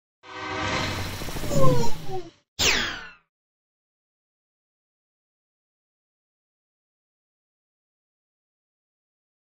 Frederator Awards Title Animation
A title bumper I made for the 2008 Channel Frederator Awards with a Meteropolis motif.